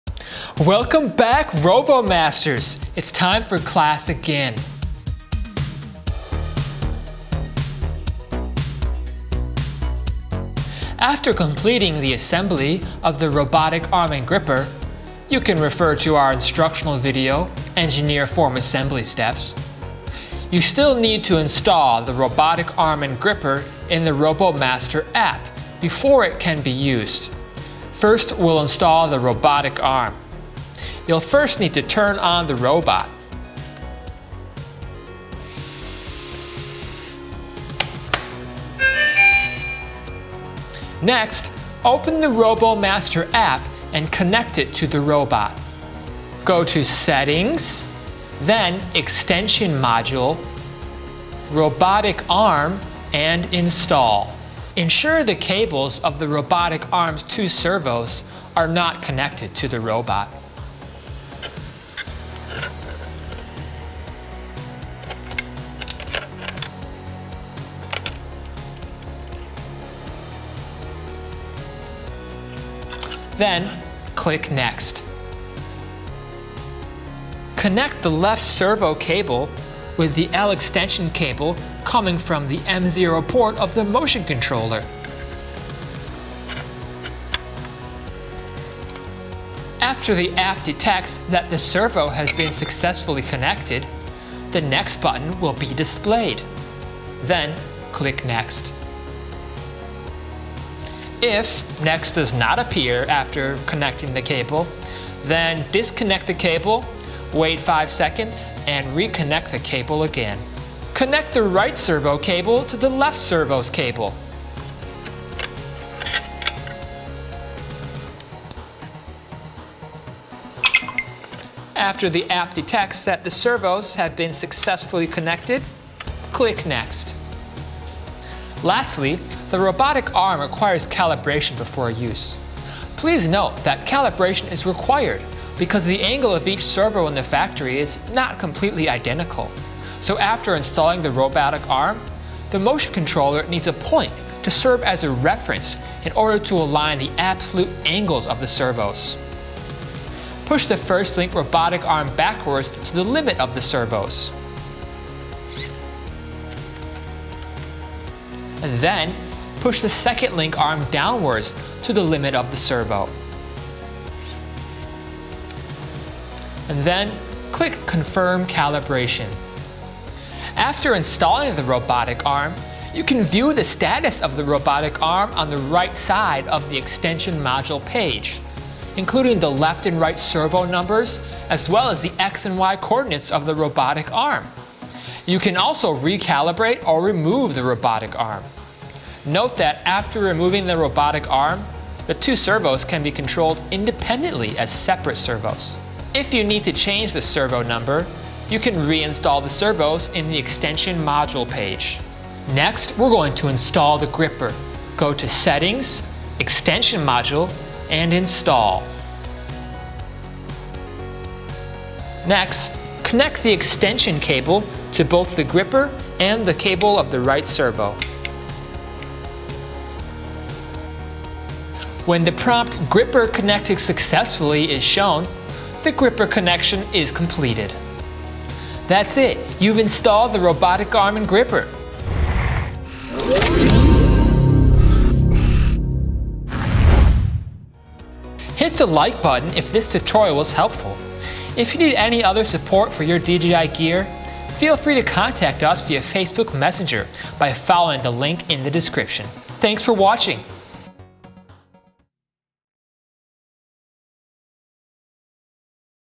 RoboMaster EP/ EP Core | How to Install the Robotic Arm and Gripper
It's time for class! Today we will show you how to install the RoboMaster EP Robotic Arm and Gripper.